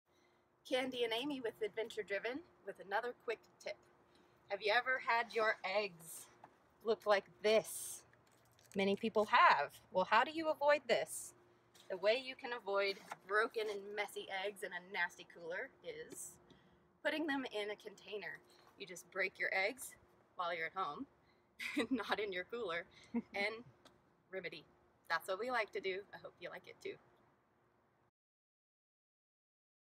Quick Tips from Adventure Driven "no more broken eggs"
Do you have a hard time keeping you eggs in the shell and not cracked while overlanding? Kande has been using this trick for 23 years now and thought she would share with you her trick.